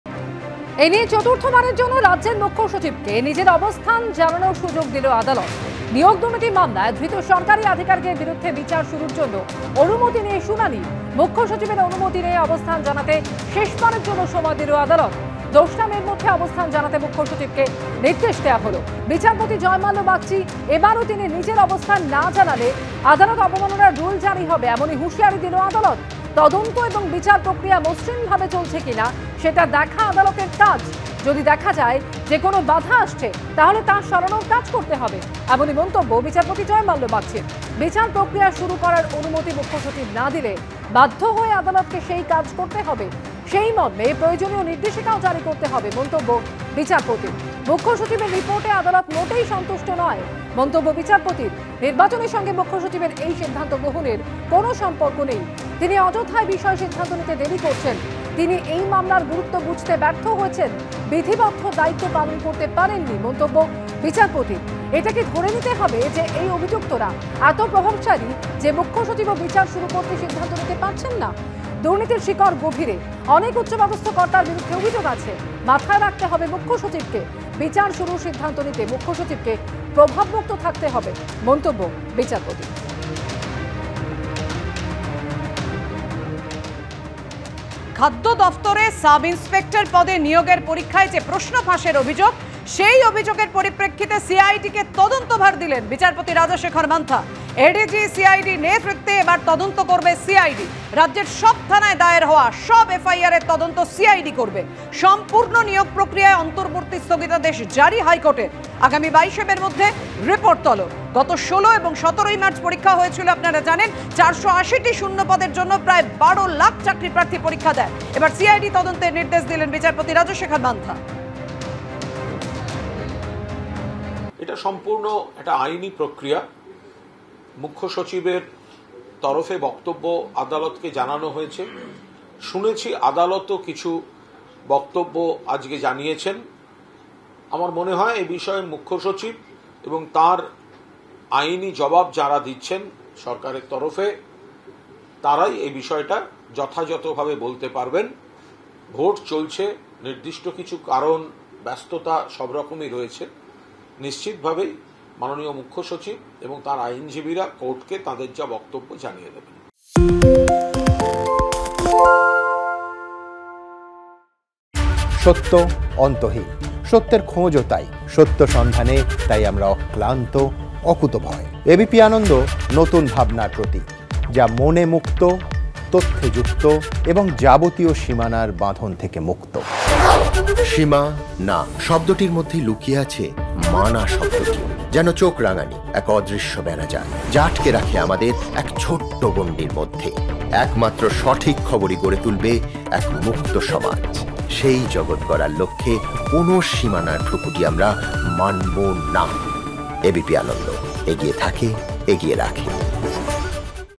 Calcutta High Court: চতুর্থবারের জন্য মুখ্যসচিবকে অবস্থান জানানোর সুযোগ দিল আদালত। ABP Ananda Live
চতুর্থবারের জন্য রাজ্যের মুখ্যসচিবকে নিজের অবস্থান জানানোর সুযোগ দিল আদালত। নিয়োগ দুর্নীতি মামলায় ধৃত সরকারি আধিকারিকদের বিরুদ্ধে বিচার শুরুর জন্য অনুমতি নিয়ে শুনানি। মুখ্যসচিবের অনুমতি নিয়ে অবস্থান জানাতে শেষবারের জন্য সময় দিল আদালত। ২ মে -র মধ্যে অবস্থান জানাতে মুখ্যসচিবকে নির্দেশ বিচারপতি জয়মাল্য বাগচীর। এবারও তিনি নিজের অবস্থান না জানালে, আদালত অবমাননার রুল জারি হবে, হুঁশিয়ারি আদালতের